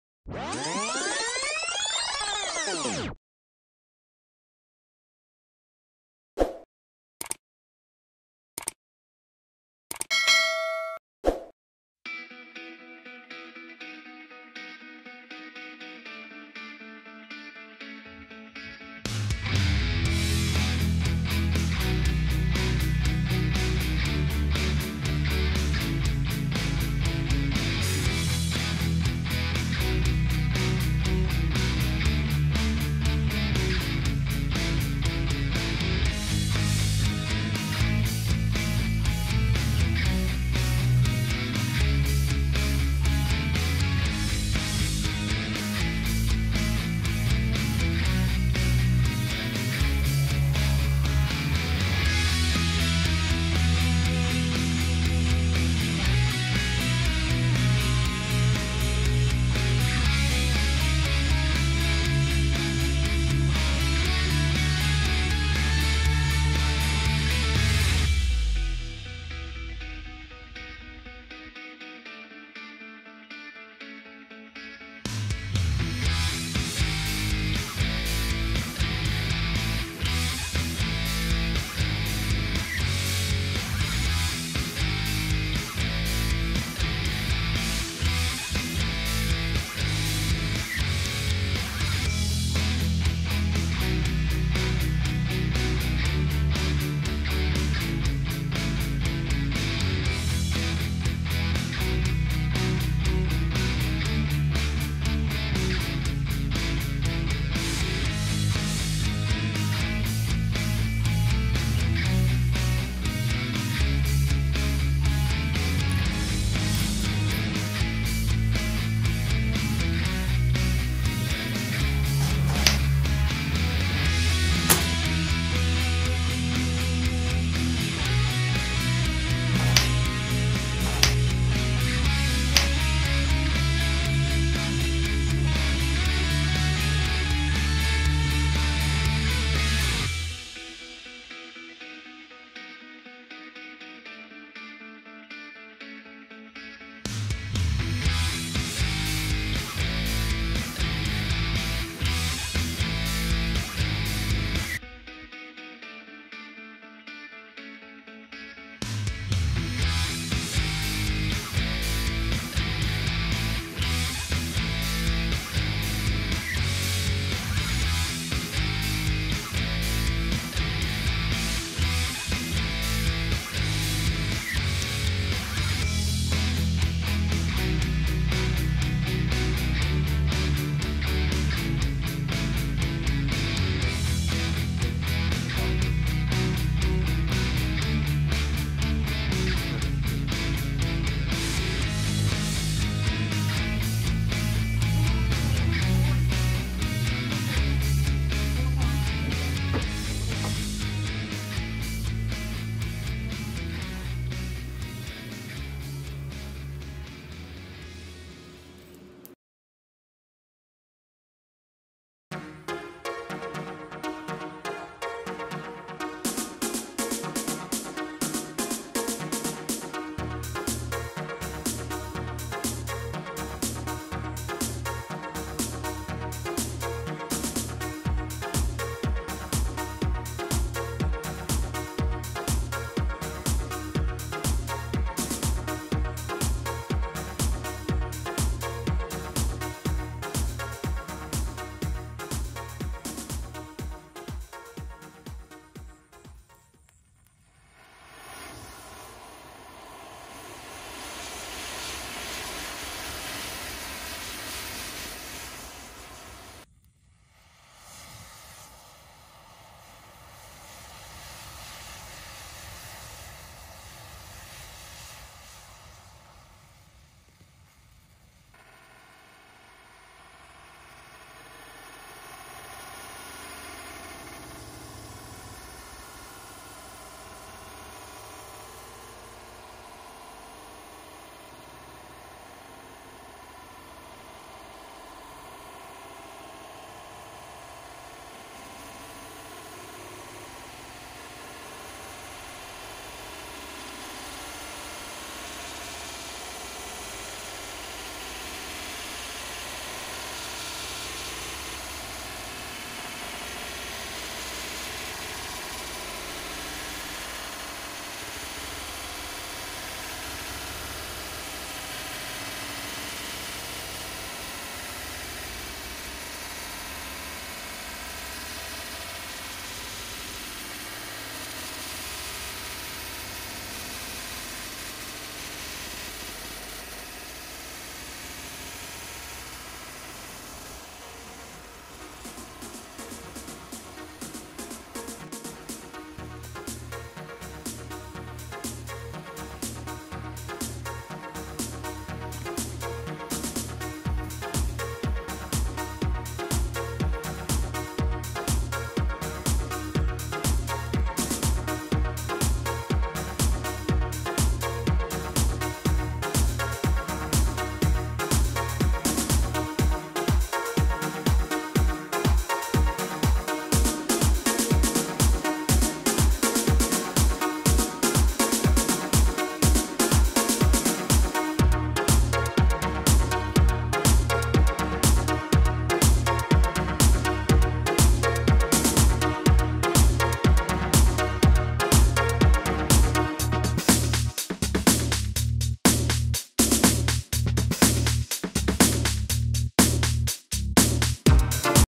FLY TO THE MOON !!🚀🚀 This Rocket was made from 100.000 matchstick for 2 weeks.
Would you like to fly to the moon on a rocket made of matchsticks with us? Okay let's go...!
What do you think of the video? I would like to thank all of you to do with this post. I do think it would have been a wonderful thing with my team.
We hope you will like it !